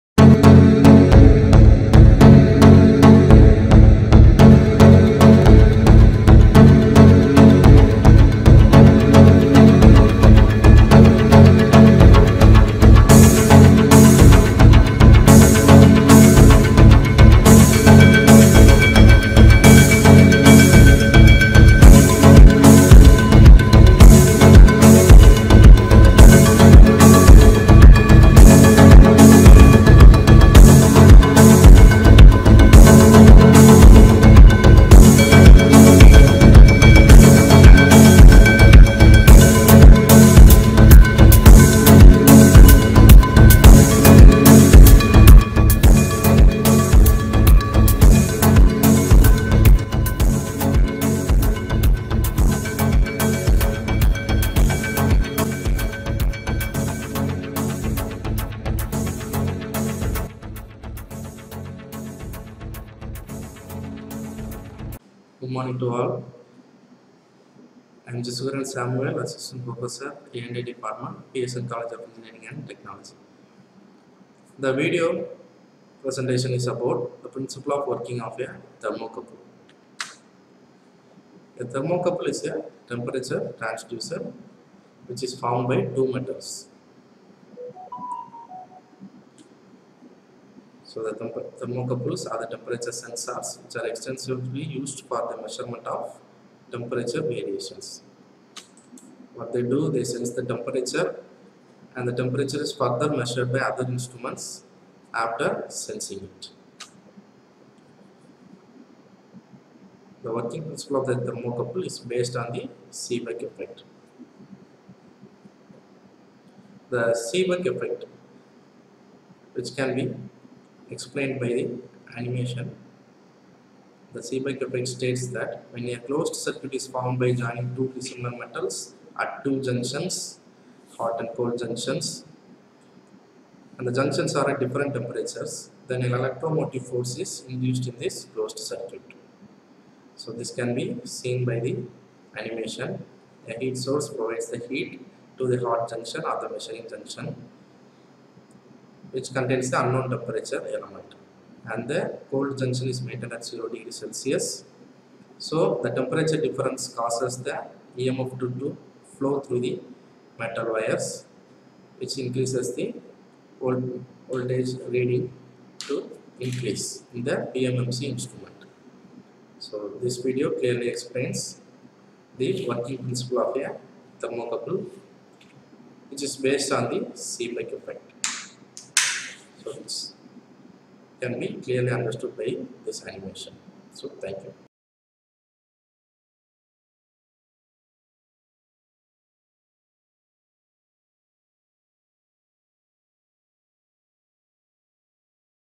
PRINCIPLE OF WORKING OF THERMOCOUPLE
Mr. Jesuharan Samuel
Assistant Professor
Electrical and Instrumentation Department
PSN College Of Engineering And Technology